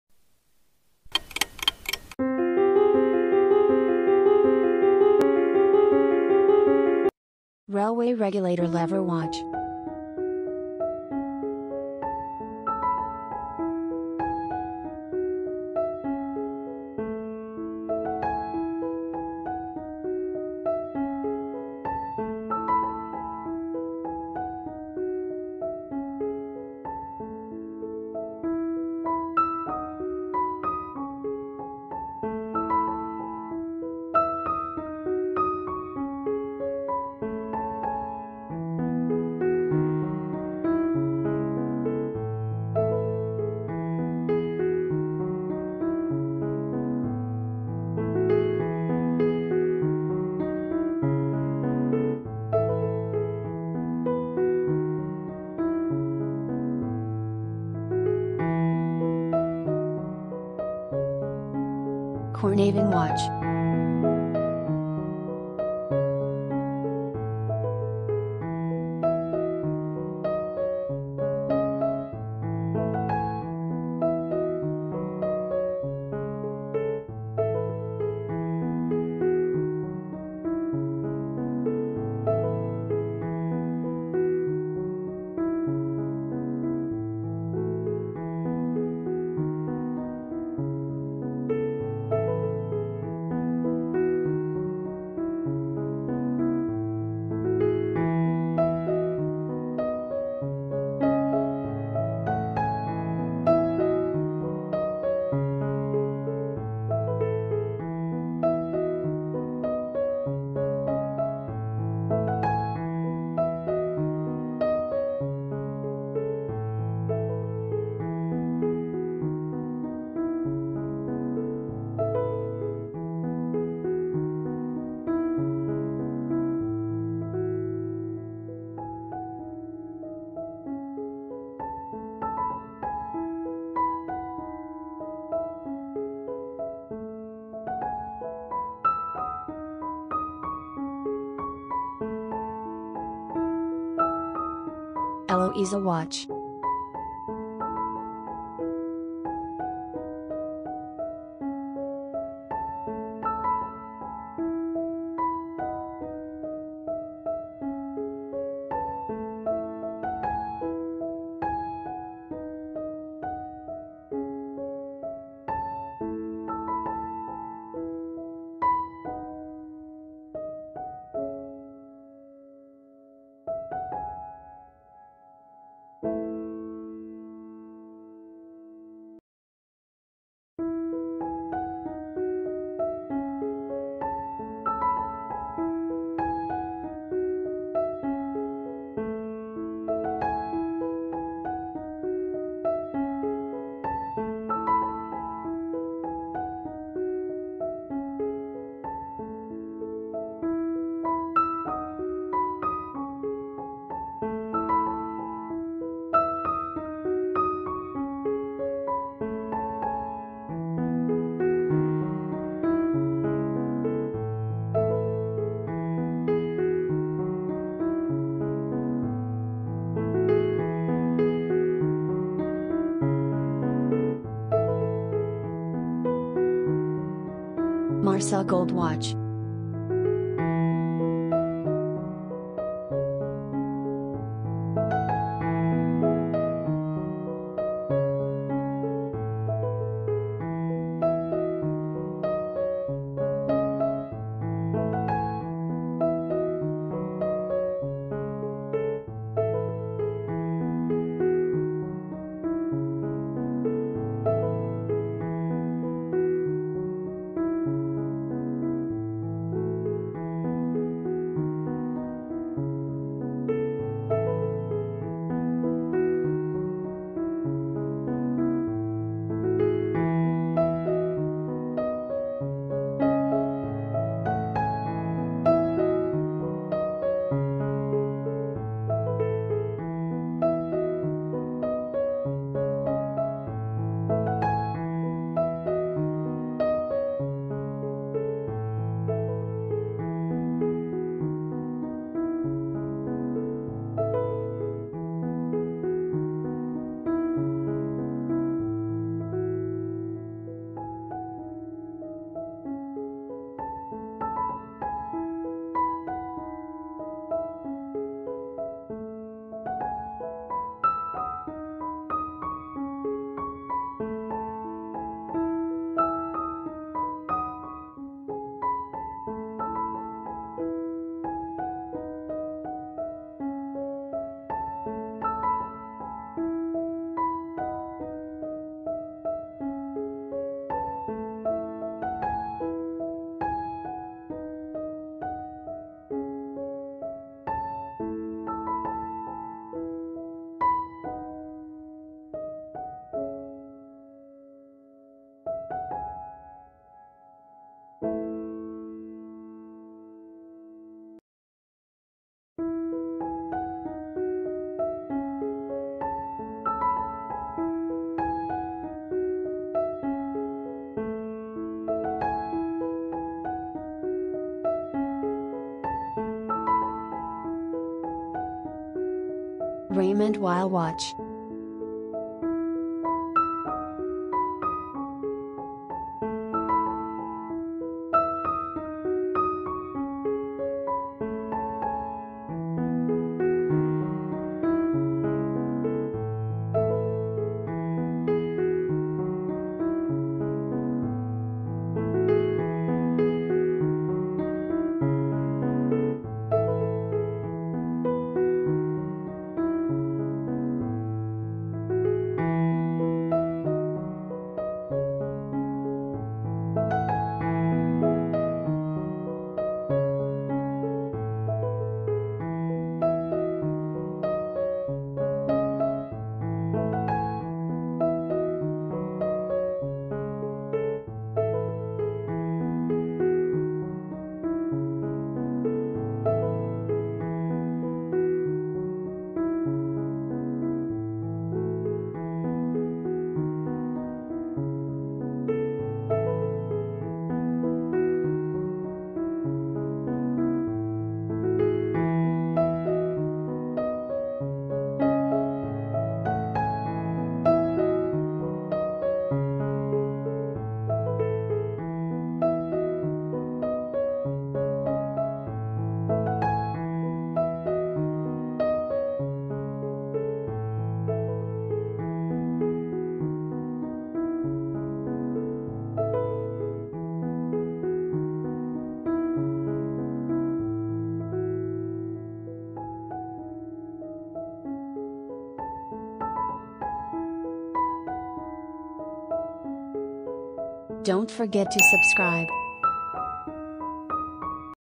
THE BEST WATCH COLLECTION FOR $ 10.000
PERFECT 5 WATCH COLLECTION

1. RAILWAY REGULATOR LEVER WATCH

2. CORNAVIN

Shock proof stainless stell back water protected

3. HELOISA

Heloisa Automatic 0609

4. MARCELLE

Quartz water resistant

Stainless stell back 3 ATM

Water resistant base metal bezel

MARCELLE

GENEVA
23 K GOLD PLATED
VF00-AX856BX

5. RAYMOND WEIL

RAYMOND WEIL
GENEVE
Othello